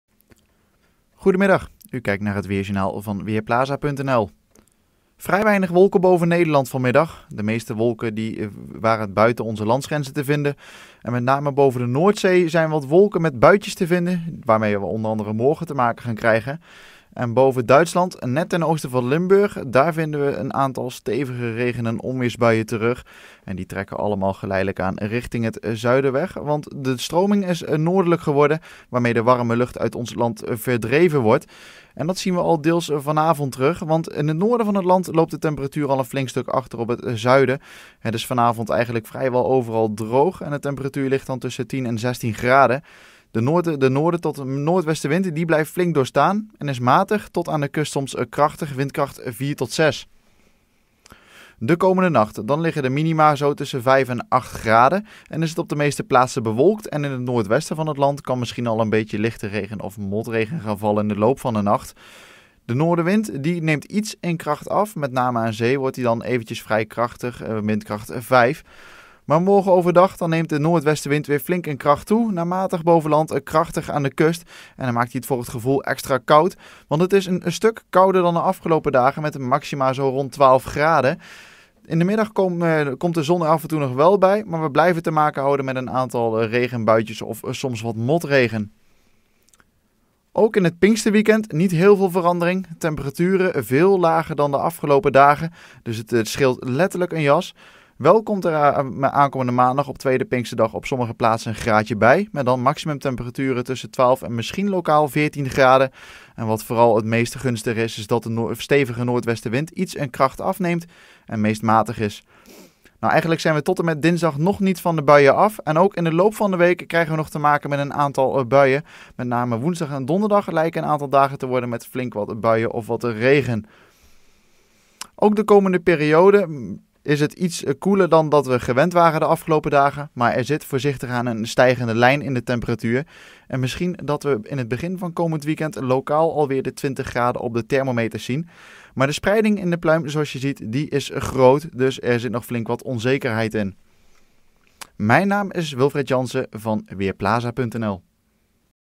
Weerbericht vrijdag 13 mei 2016: Flink kouder weer
Vanavond is het nog lang zonnig. In het zuidoosten kan nog een bui vallen maar elders blijft het droog. Morgen is er meer bewolking en in de loop van de dag vallen er enkele buien. Er waait een matige tot krachtige noordwestenwind die het erg fris maakt. De middagtemperatuur ligt tussen 10 graden op de Wadden en 13 graden in het zuidoosten van het land. Ook de pinksterdagen zien er fris en wisselvallig uit met enkele buien. wel breekt af en toe de zon door. Na Pinksteren gaat de temperatuur geleidelijk omhoog.